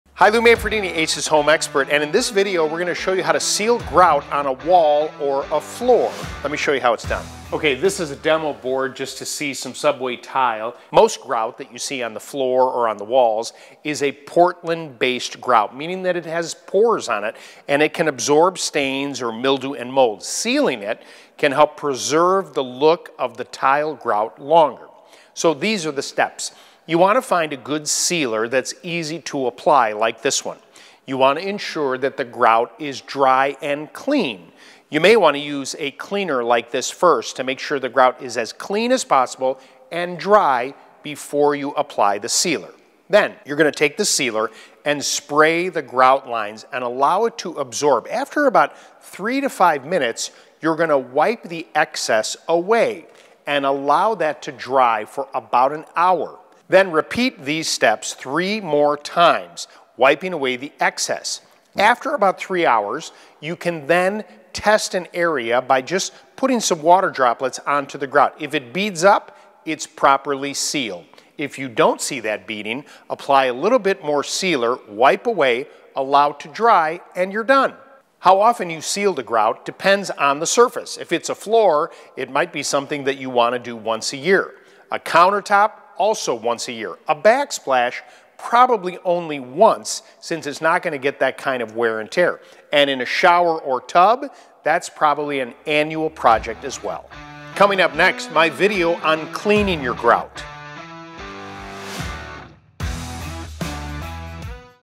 How To Seal Tile Grout - Ace Hardware
Watch as Lou Manfredini, Ace's Home Expert, shows you an easy way to seal your tile grout.